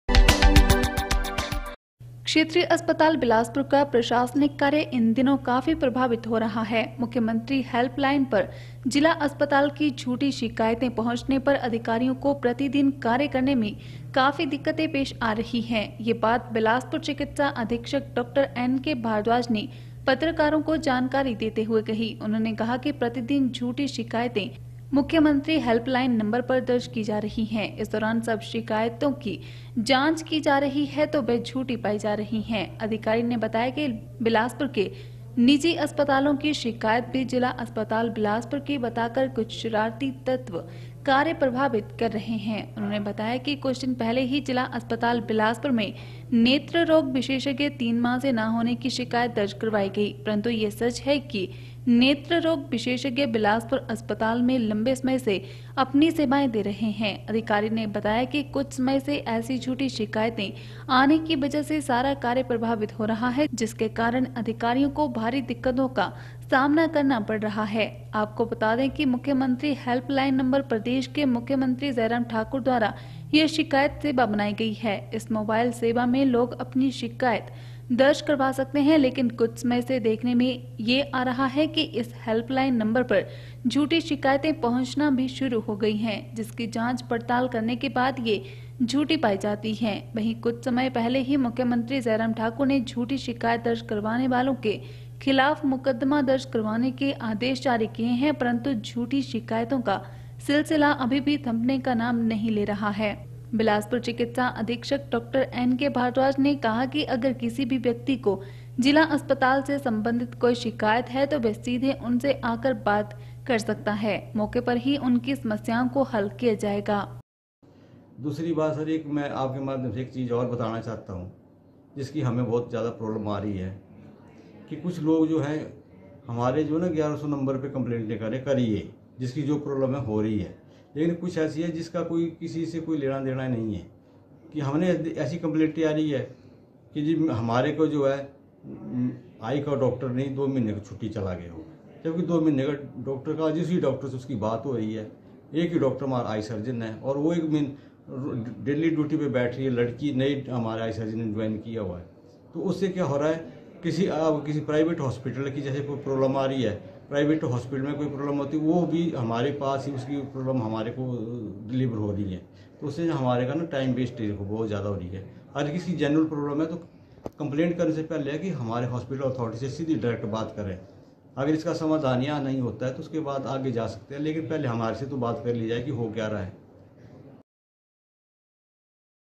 जिला अस्पताल बिलासपुर की मुख्यमंत्री हेल्पलाइन नंबर पर दी जा रही झूठी शिकायतें
क्षेत्रीय अस्पताल बिलासपुर का प्रशासनिक कार्य इन दिनों काफी प्रभावित हो रहा है। मुख्यमंत्री हेल्पलाइन पर जिला अस्पताल की झूठी शिकायतें पहुंचने पर अधिकारियों को प्रतिदिन कार्य करने में काफी दिक्कतें पेश आ रही है। यह बात बिलासपुर चिकित्सा अधीक्षक डाॅ. एनके भारद्वाज ने पत्रकारों को जानकारी देते हुए कहीं। उन्होंने कहा कि प्रतिदिन झूठी शिकायतें मुख्यमंत्री हेल्पलाइन नंबर पर दर्ज की जा रही है।